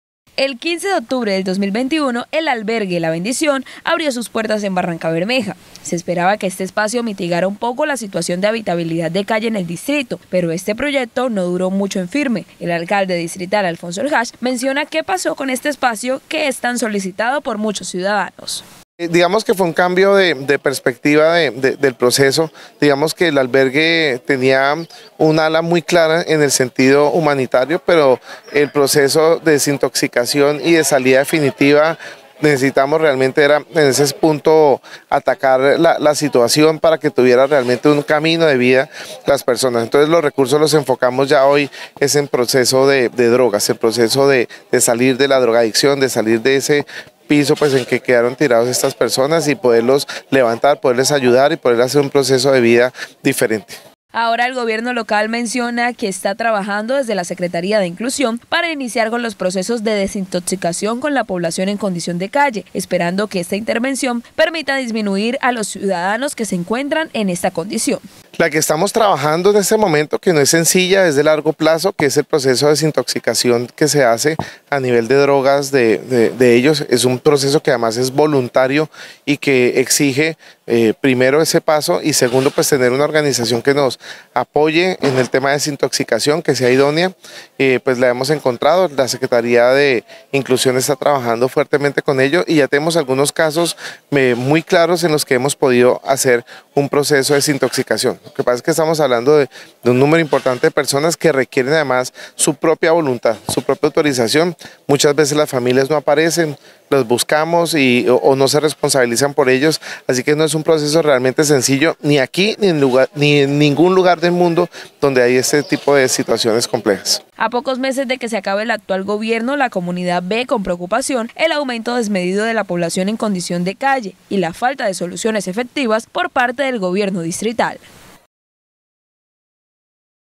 ¿Qué pasó con el albergue para habitantes de calle?
¿Qué está haciendo el Gobierno local para controlar la situación con los habitantes de calle y las nuevas ollas de microtráfico que se están formando en Barrancabermeja? ¿Qué pasó con el proyecto del albergue “La Bendición”, el alcalde Alfonso Eljach responde: